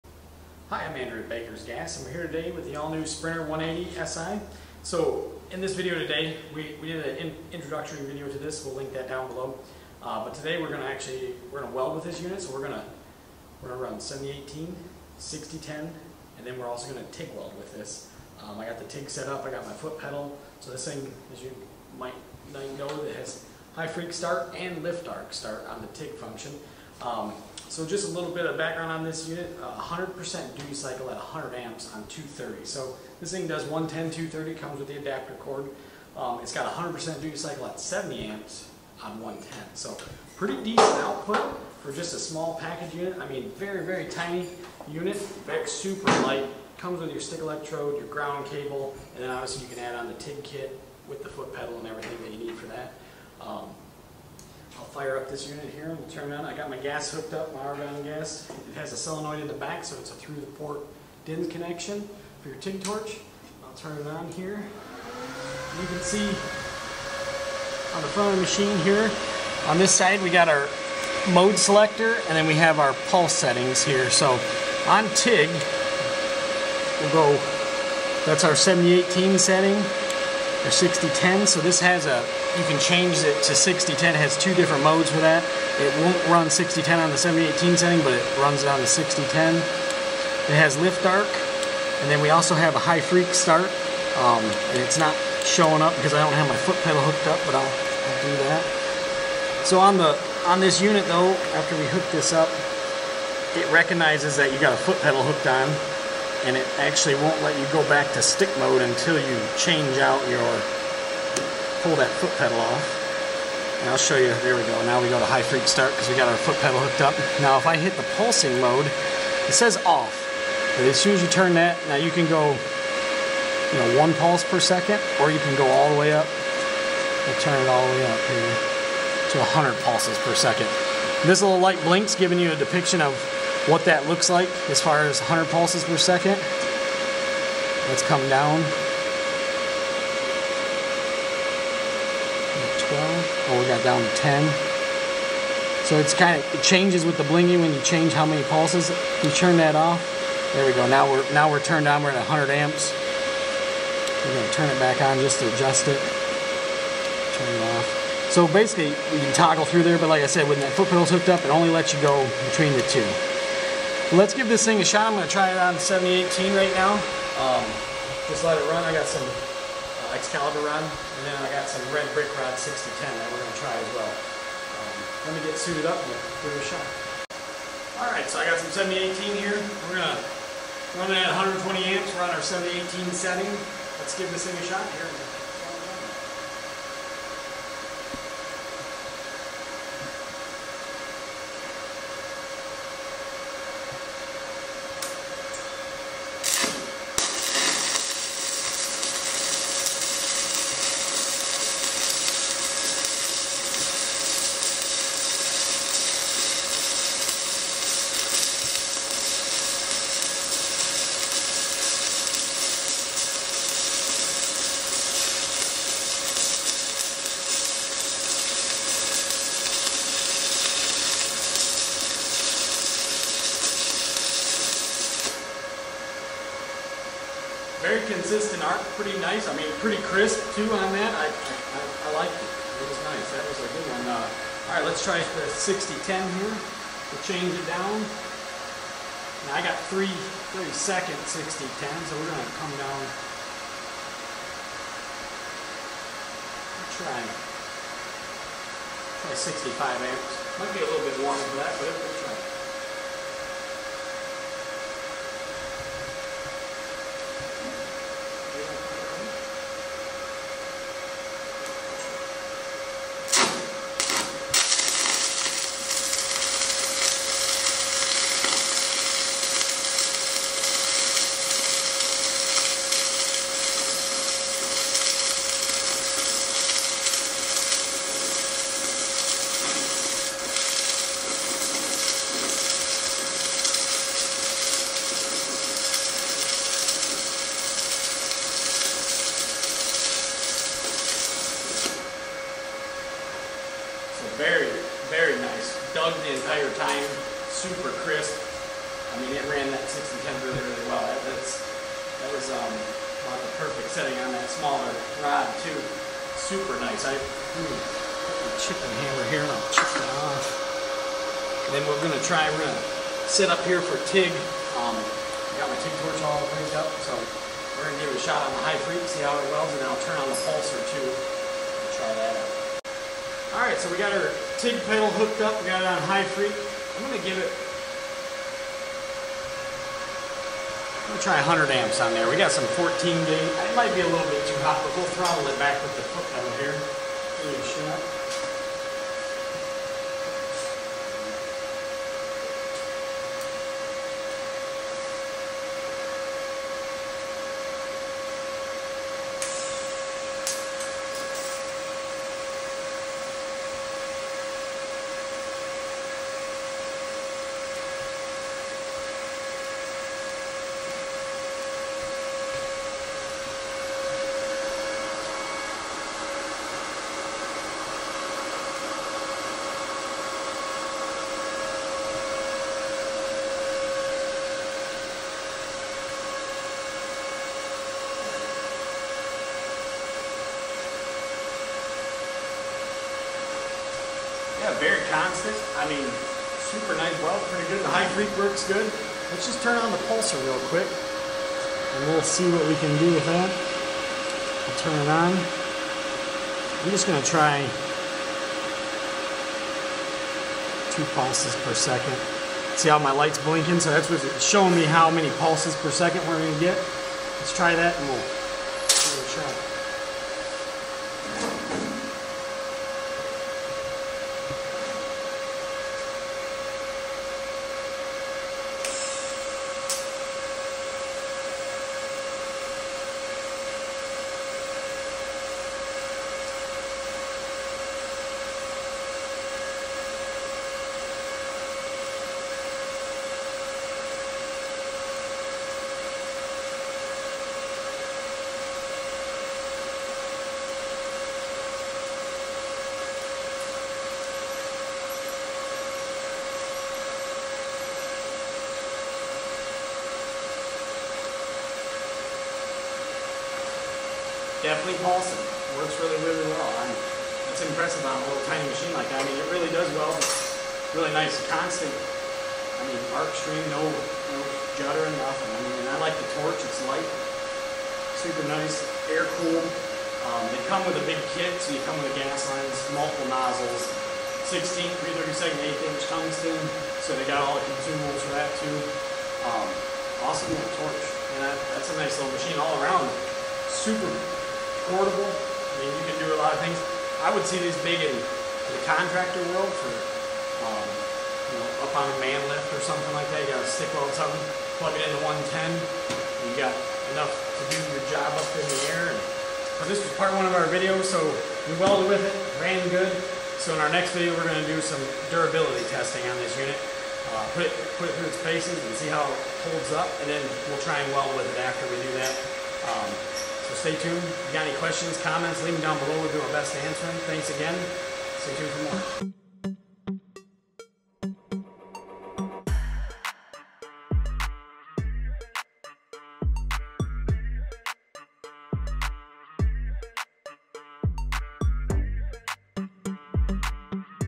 Lincoln Sprinter 180 Welding Review Stick and TIG welding demo
In Part 2 of our deep dive into the Lincoln Sprinter 180Si, the team at Baker’s Gas puts this powerful dual-process machine to the test with real-world Stick and TIG welding demos.

🔥 Overview

Stick welding with 6010 (Crisp Arc mode) and 7018 (Soft Arc mode)

TIG welding with HF start and pulse settings

Close-up look at the user-friendly screen and how to switch modes

Arc starts, bead control, and performance insights from our welder Andrew

Whether you’re in the shop or in the field, this compact 180-amp inverter proves it’s built for serious performance. Watch the puddle, hear the arc, and see why the Sprinter 180Si is built for both pros and learners.